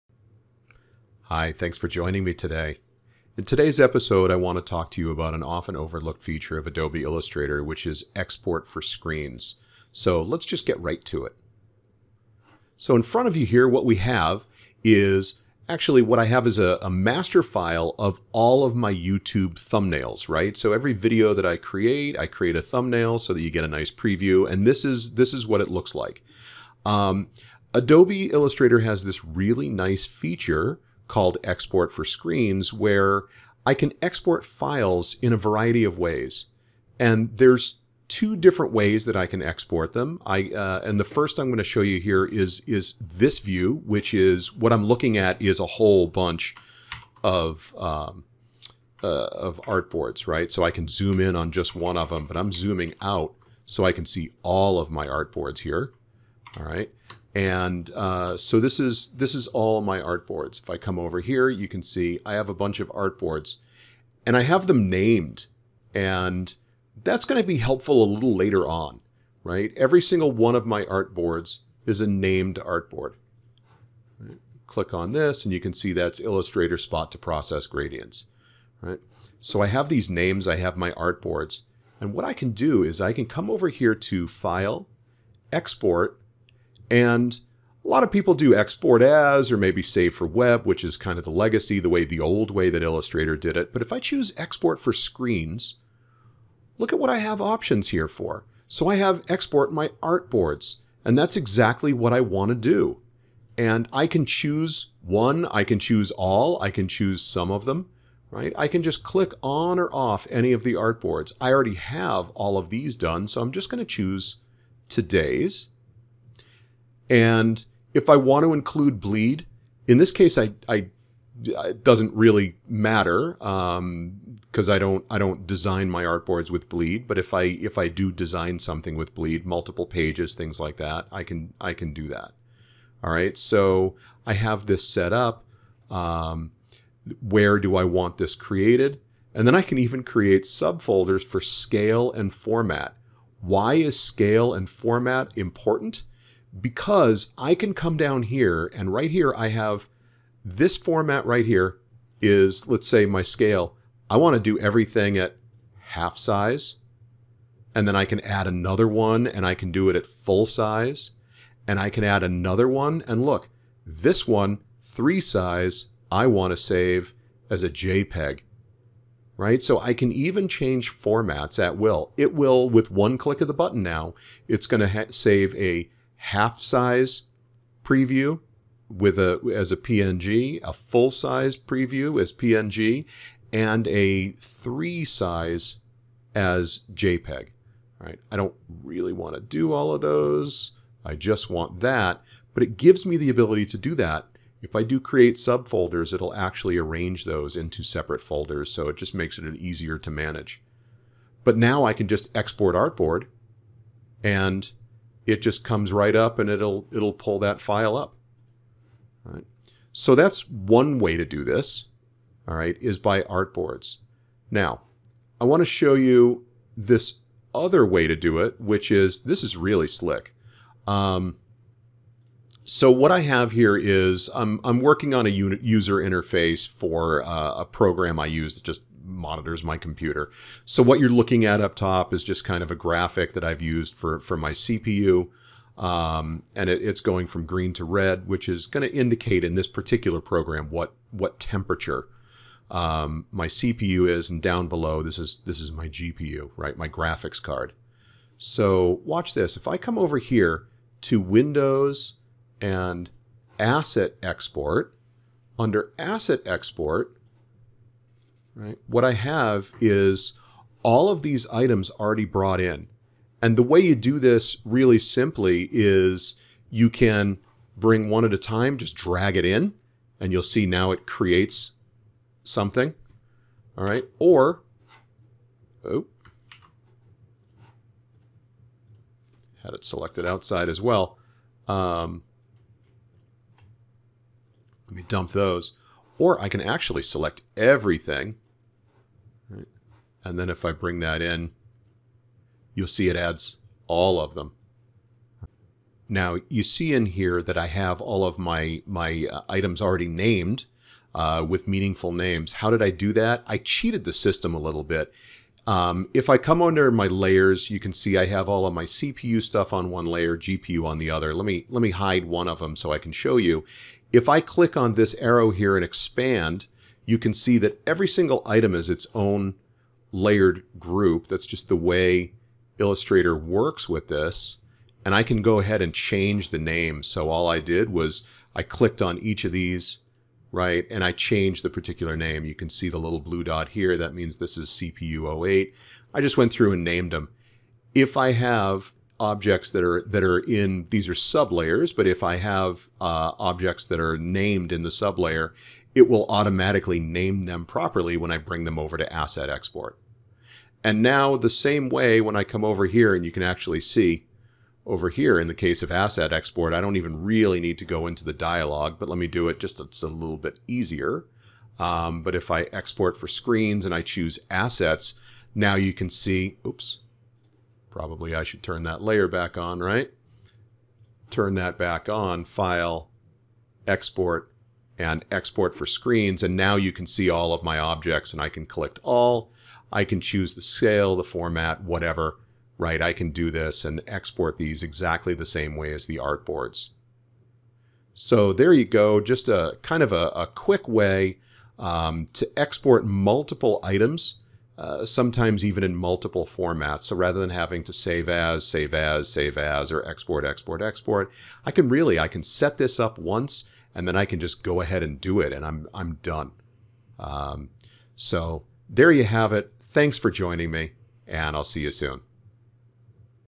Adobe Illustrator - Export for Screens
A quick overview of one of Adobe Illustrator's often-overlooked features: Export for Screens. If you need to export more than one discrete object at a time, or need to export to multiple formats at a time, this feature can be a real time-saver!